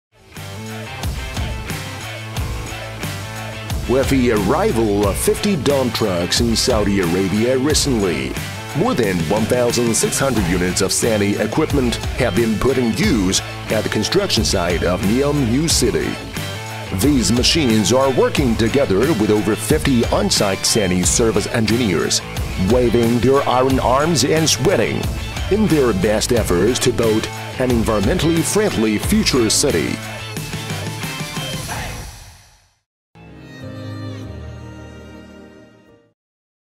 SANY successfully ships fifty dump trucks to Saudi Arabia
The 7th episode of "SANY News" series takes you to the NEOM New City in Saudi Arabia. In this episode, SANY successfully ships fifty dump trucks to Saudi Arabia. To date, over 1600 SANY machines have been put into use at the NEOM new city construction site.